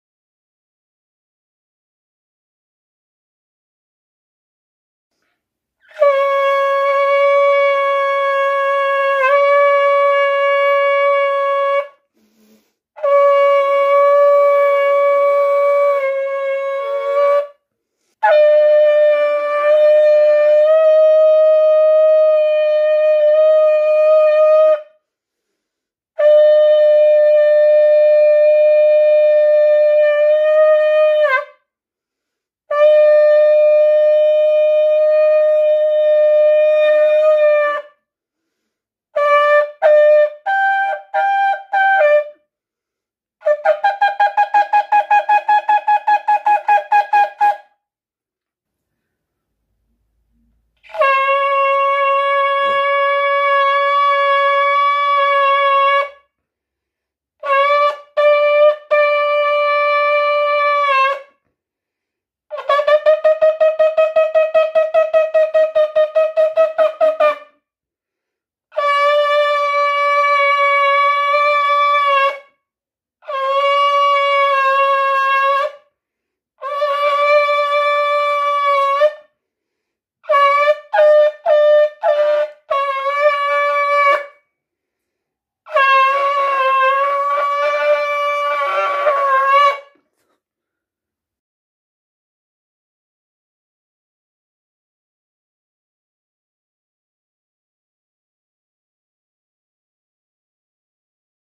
Yom Terou'ah - Fête de la Trompette
En ces jours de la fête du Shofar, je désire bénir Dieu avec vous.  Le Seigneur nous appelle à une sainte convocation.  Réunissons-nous pour le louer et l’adorer, soyons prêt, Yeshua Revient Bientôt.